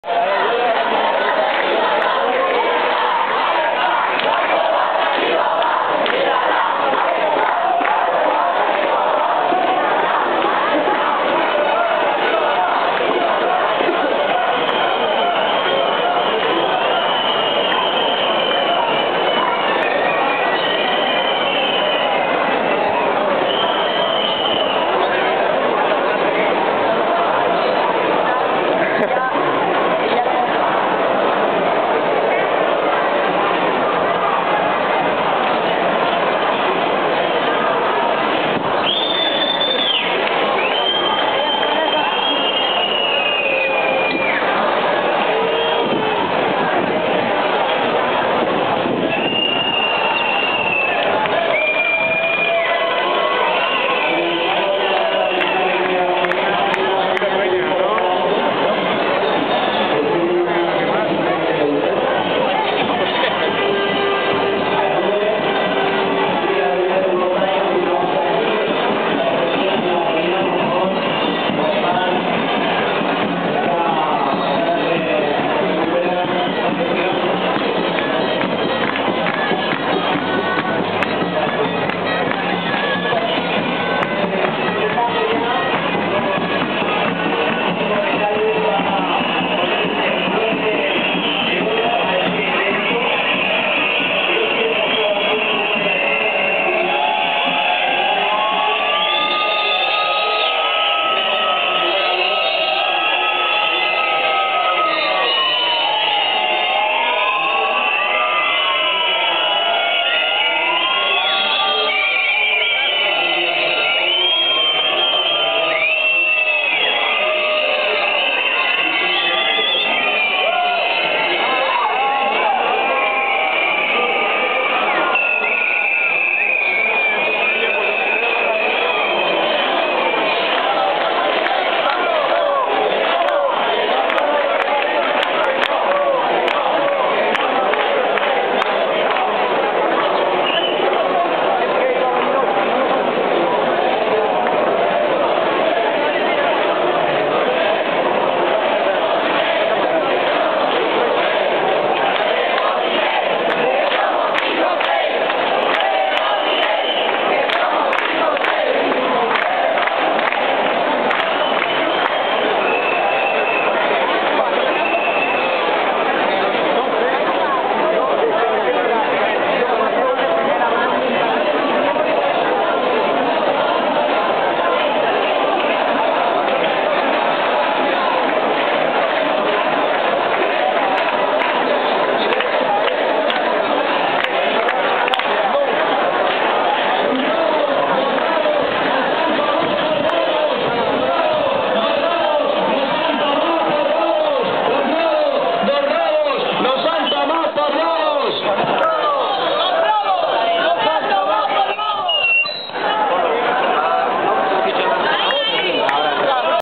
Manifestación 19-J Valladolid
Manifestación del Movimiento 15-M Valladolid contra el Pacto del Euro, desde su cabecera en la Plaza Zorrilla hasta la Plaza Colón, su punto de salida, ocupando toda la Acera de Recoletos.